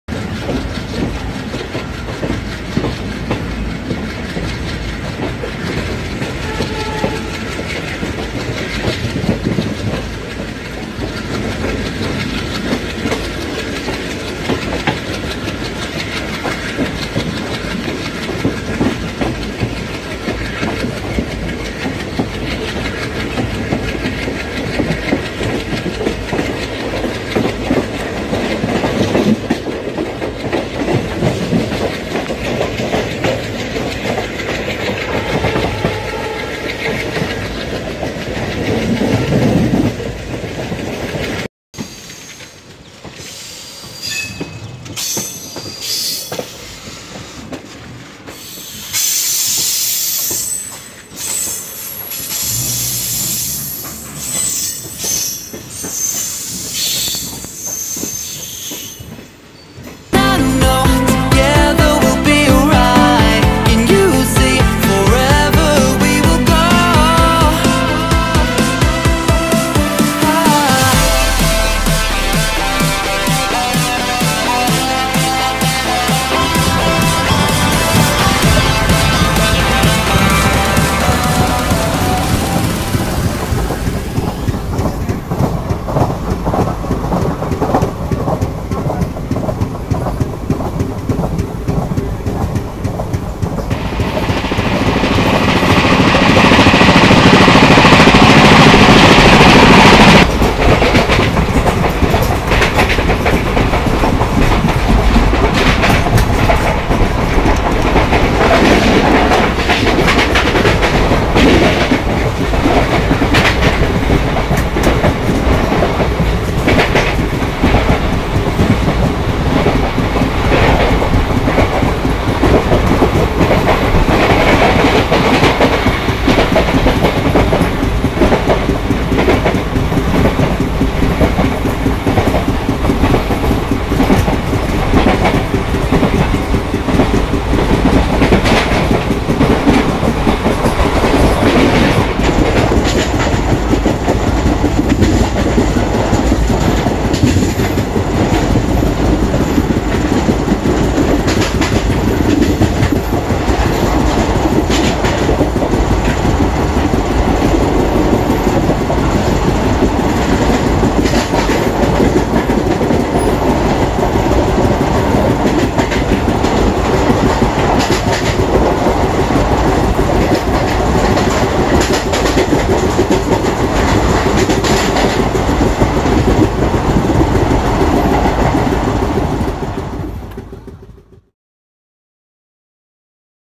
NIGHT JOURNEY TRAILER || Gorakhpur_Lucknow_kanpur route Show down
This video is a trailer of Gkp_lko_cnb night version

soon The full version will release, editing is on.